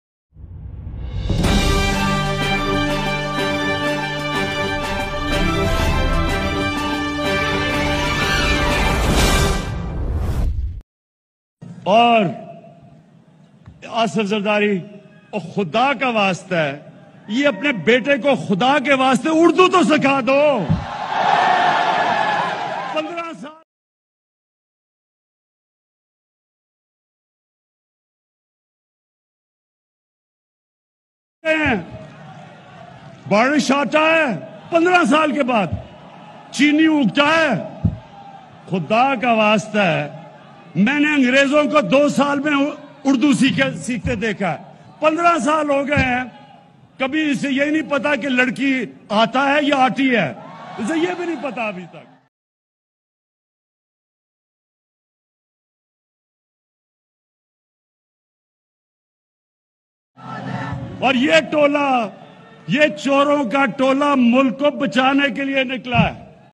Imran Khan aggressive speech about oppostion/ Imran Khan did bilawal mimicry in live speech today
this video is about Imran Khan who did mimicry of bilawal butto zardari jab Barish ata Hain to Pani ata Hain Pakistan main cheni ugta Hain he also request asif zardari to plz learn Urdu to bilwal butoo zardari because yesterday bilwal said kanpay tank Rahi hain which is very funny Imran Khan said that biwal has not learned Urdu from last 15 years and he said that I have seen British people who  learn Urdu in 2 years he criticise fazal ur Rahman and zardari also he said when nab call him he feel pain in his backbone